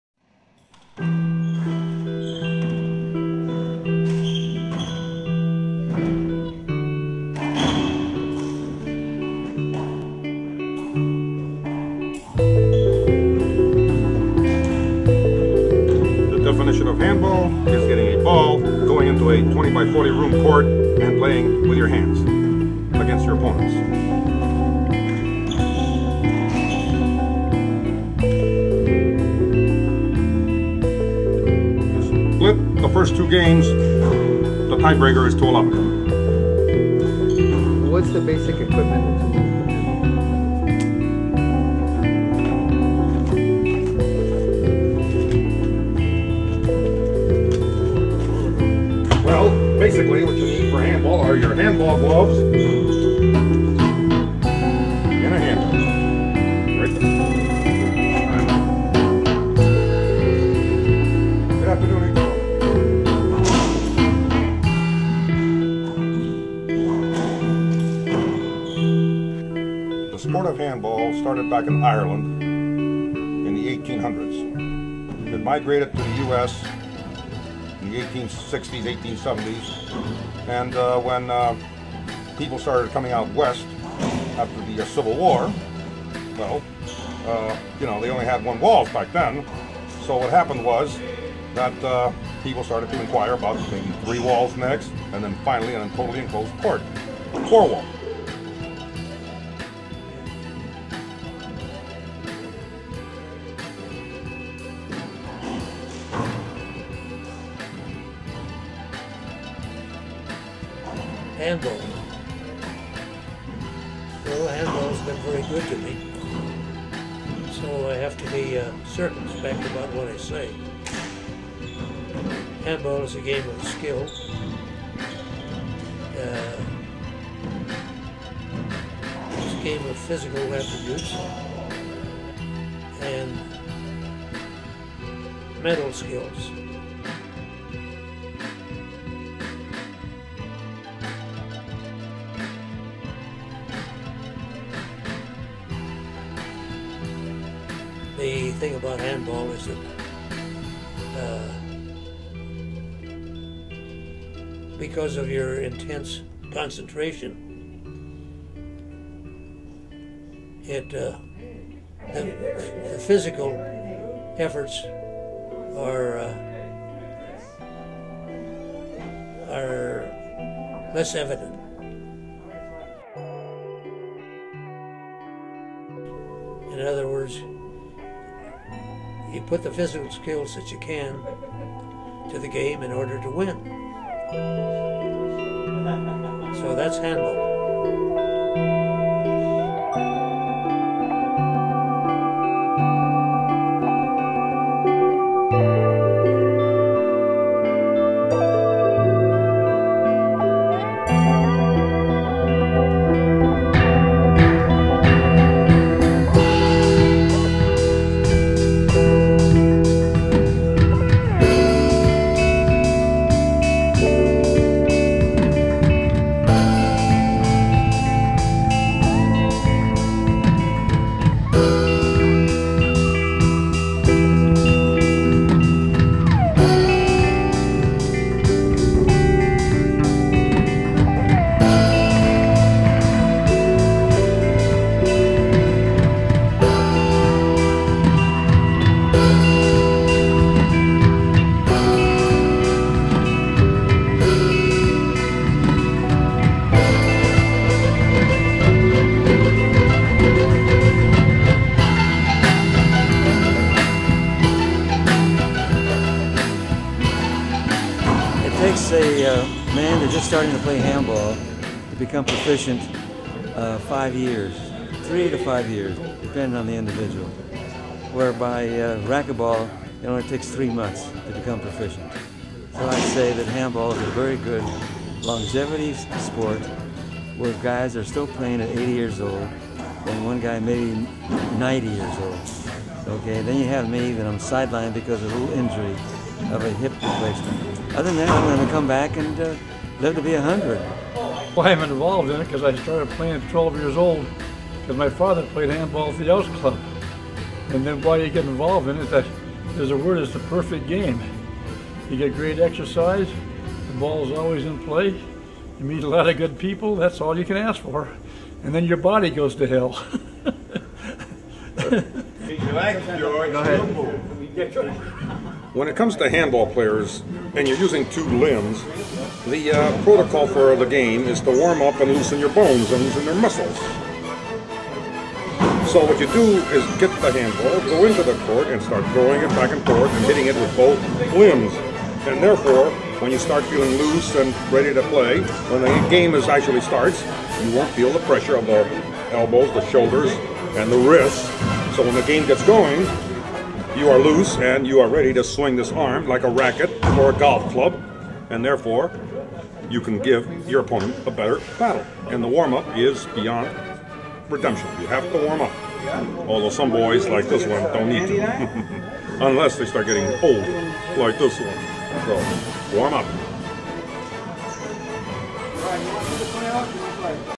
Handball Exhibition at the LAAC PT 1
The question as to what is handball and how you play it is defined by a few veteran players from the LAAC.